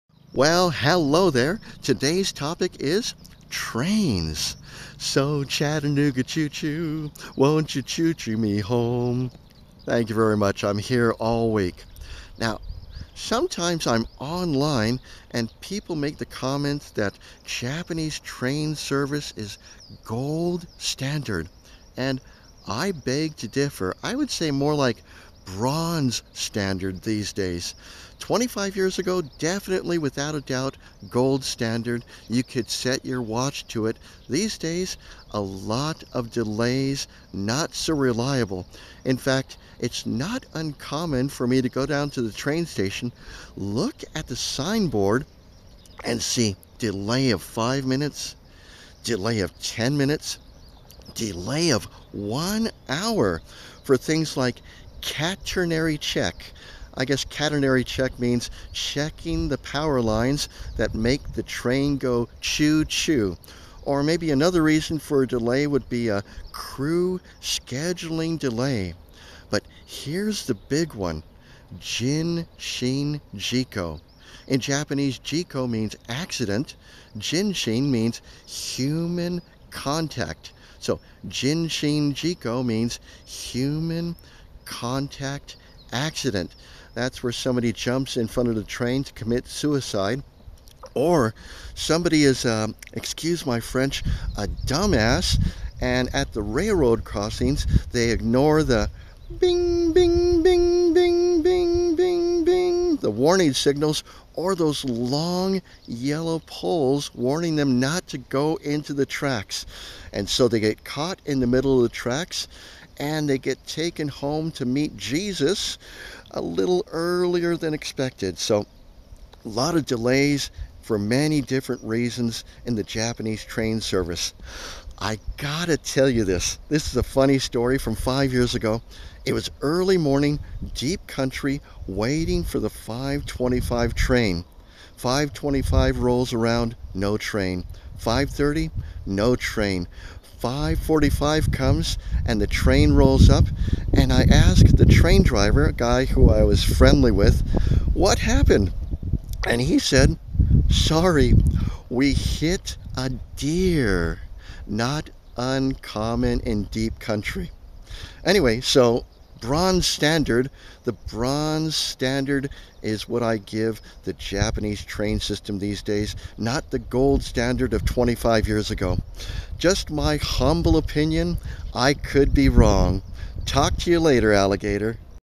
Are Japanese Trains ”Gold Standard”? (3 minutes)
In my humble opinion, Japanese trains are not as reliable in comparison to past years.
There are regular delays for weather, crew scheduling, power line checks,
and ”human contact accidents,” ”Jinshin Jiko” 人身事故. What used to be ”Gold
Standard” is now, a less reliable, ”Bronze Standard.”